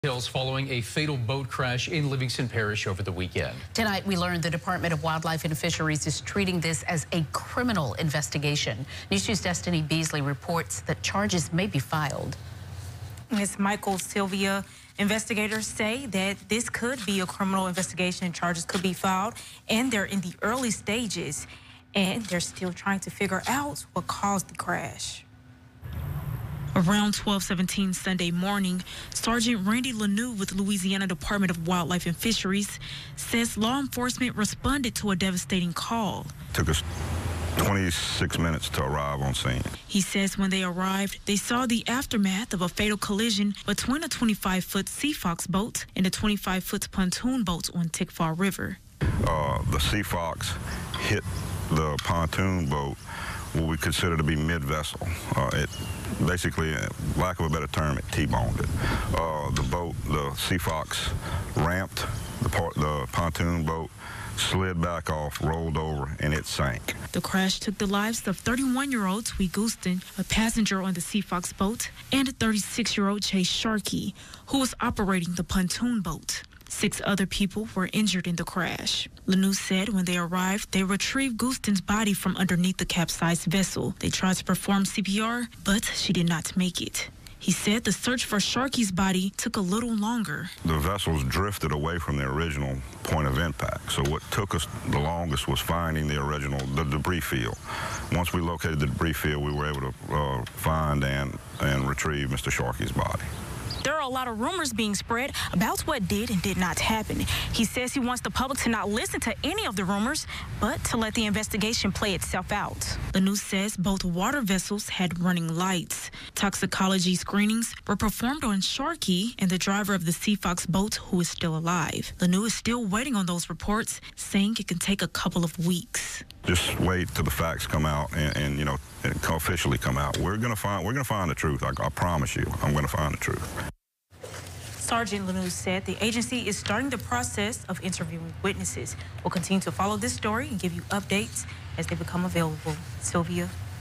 New details revealed on weekend's fatal Tickfaw River boat crash; charges could potentially be filed
The Louisiana Department of Wildlife and Fisheries said Tuesday that it is treating the weekend's fatal boat crash on Tickfaw River as a criminal investigation.